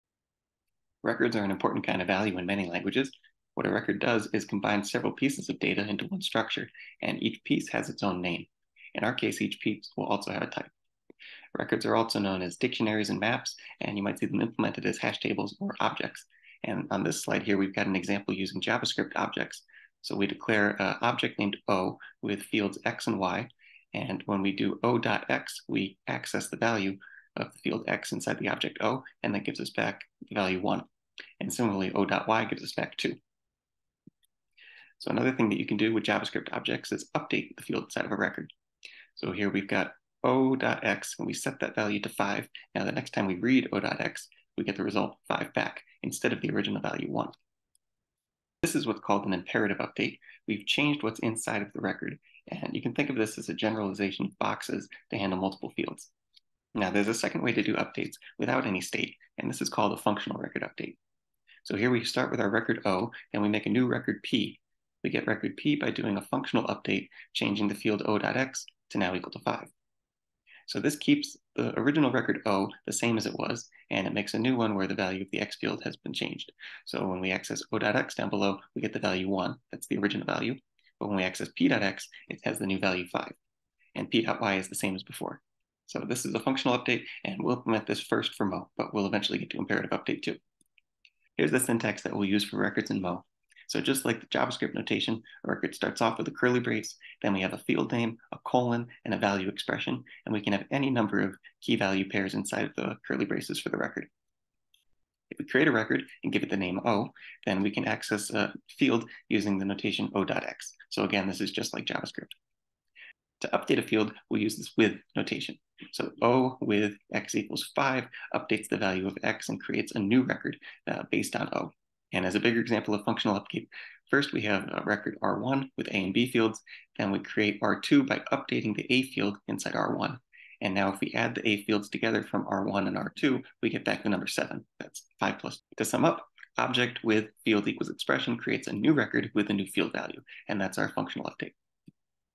Records 1 — intro
Records: expressions that create records, get record fields, and set record fields.